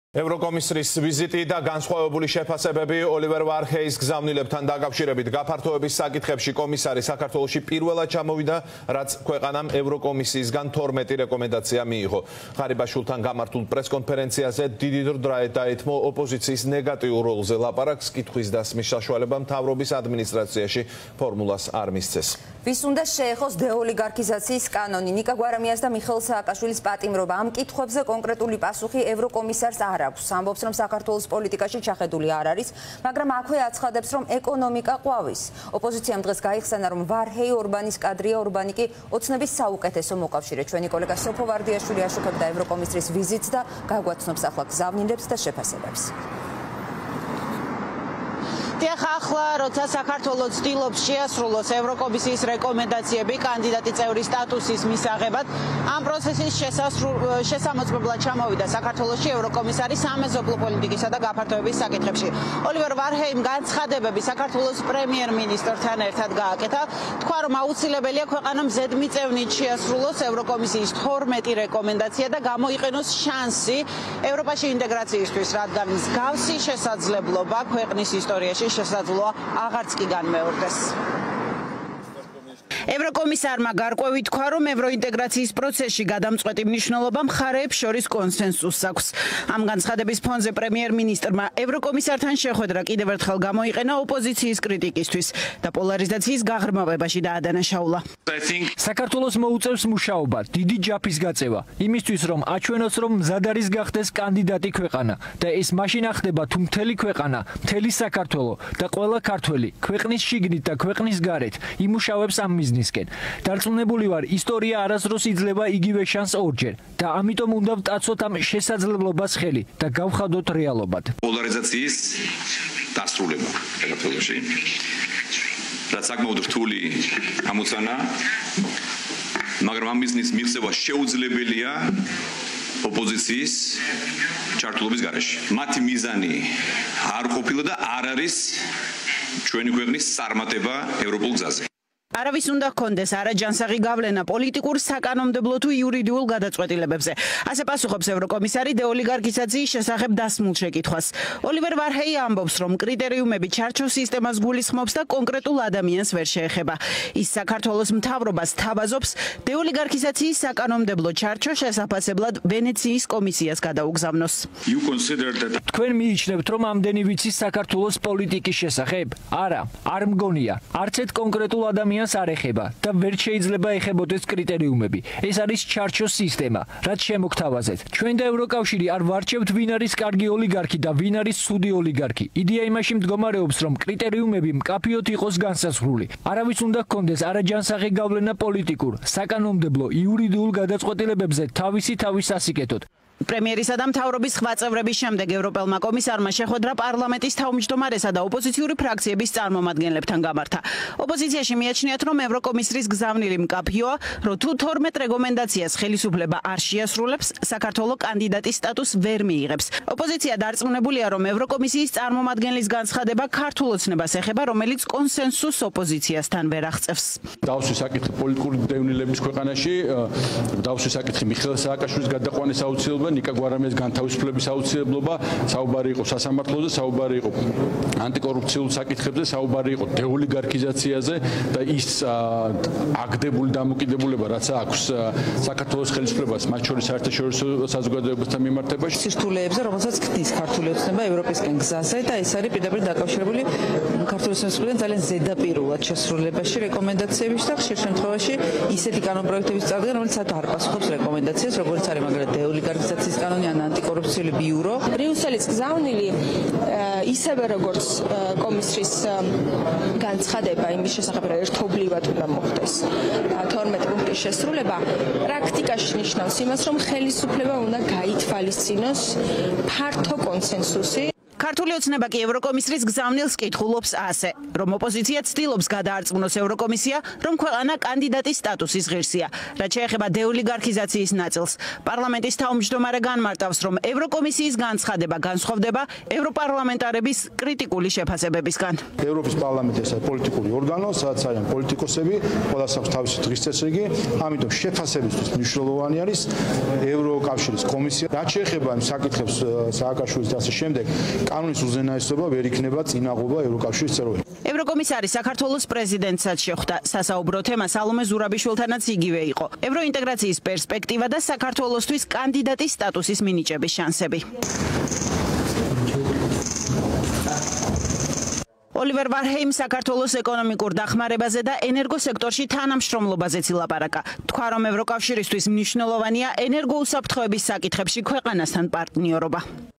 ვინ როგორ გაიგო ევროკომისრის გზავნილები?
▪️ ევროკომისრის ვიზიტი საქართველოში და განსხვავებული შეფასებები — ვინ როგორ გაიგო ოლივერ ვარჰეის გზავნილები